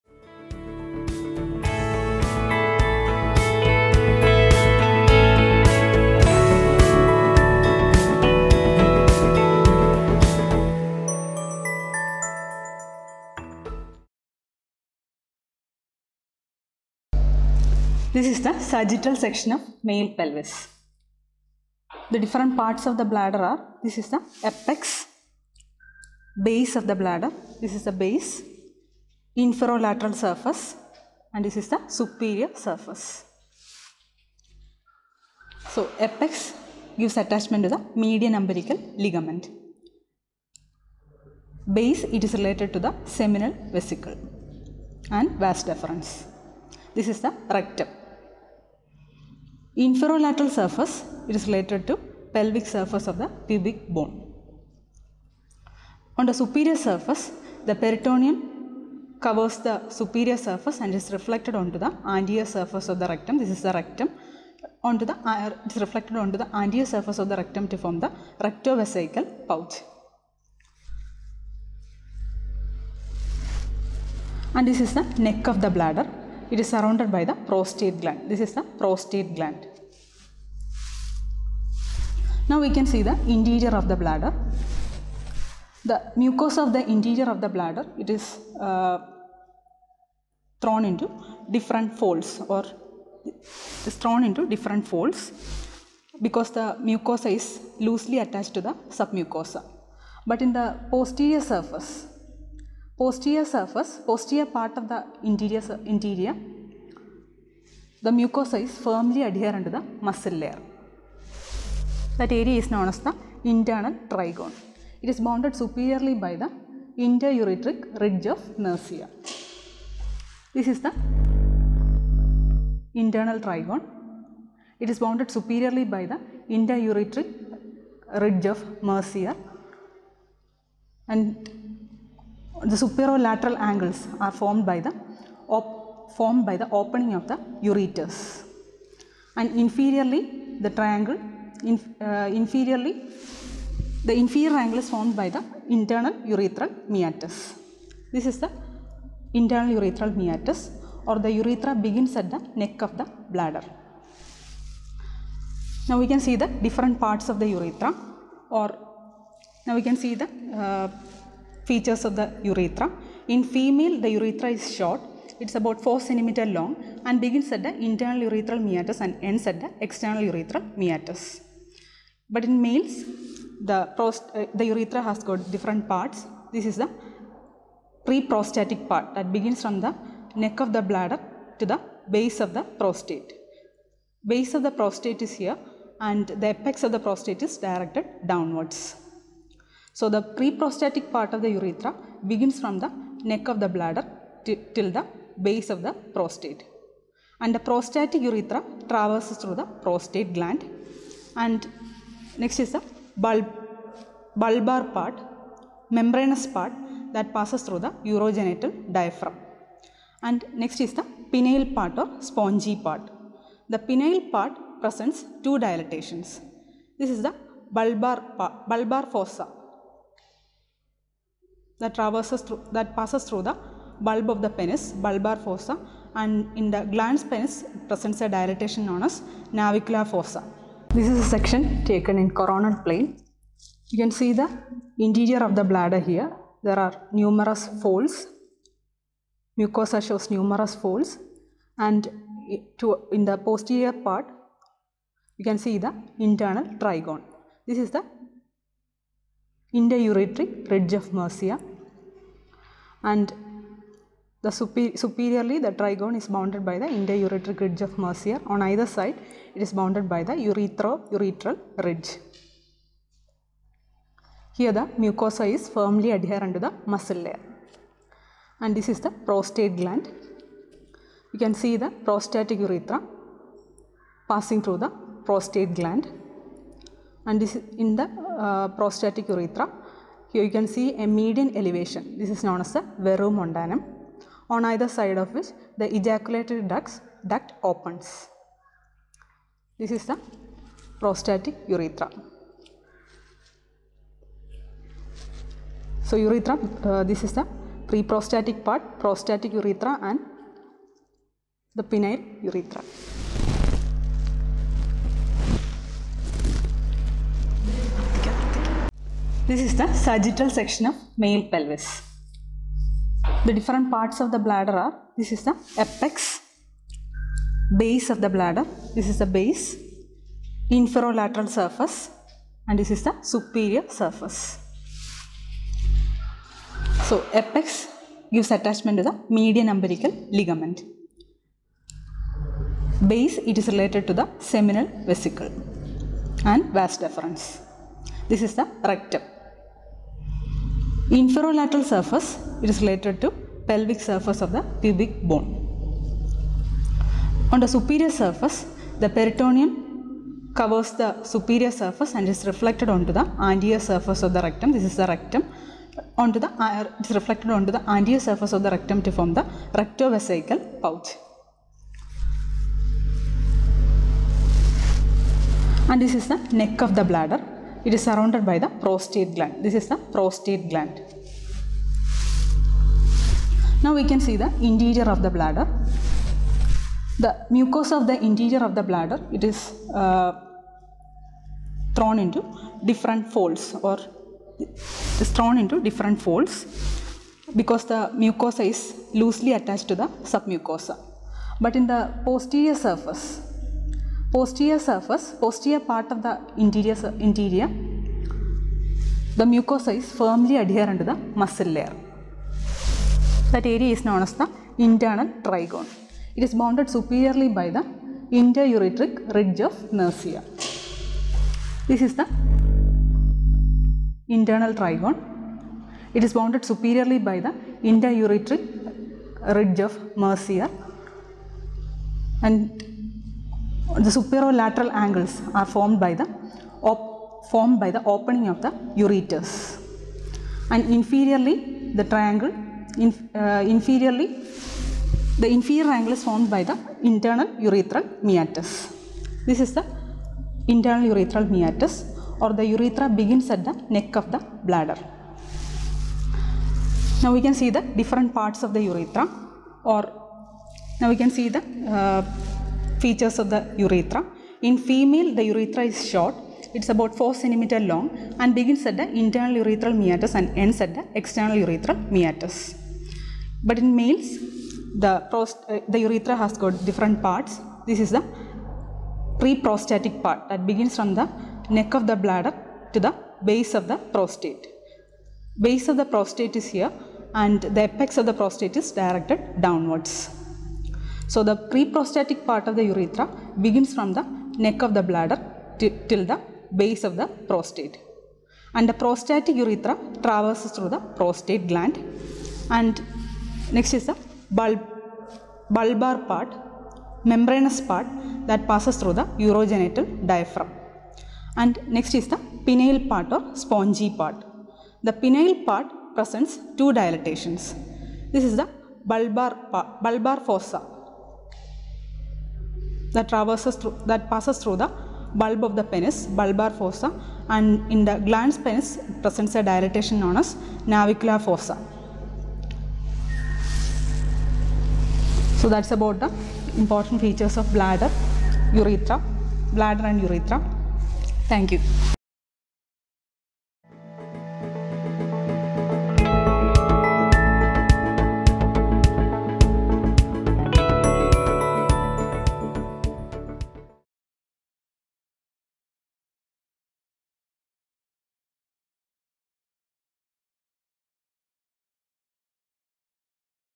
Pelvis-4: Urinary bladder & Urethra
Anatomy of Urinary bladder and urethra.
Presenting parts and relations of urinary bladder
Male urethra, extent and internal features